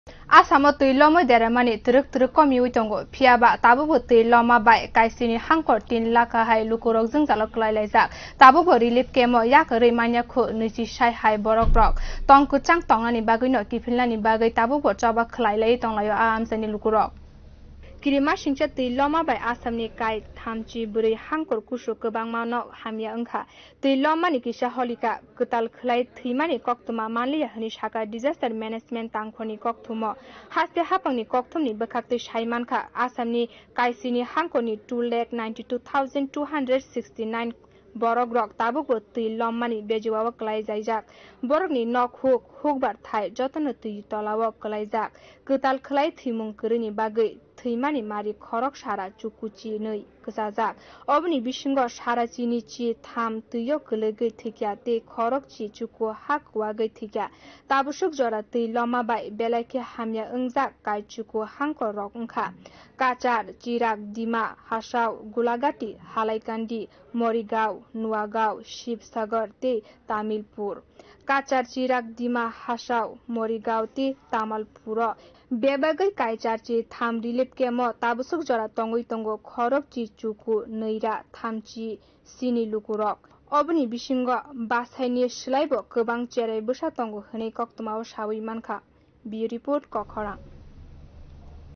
Assam o twi lomwi deramani twrwk twrwk komiwi tongo
Assam o twi lomwi deramani twrwk twrwk komiwi tongo. phiyaba tabukbo twi lomma bai kai sini hangkhor 3 lakh ahai lukurog jwngjalo kwlaijak kho. Tabukbo Relief camp yakarwi manyakhu nwichisai hai borok rog. Tong kwchang langmao kiphilna bagwi tabukbo choba khwlai laiwi ma tonglai jakho borog.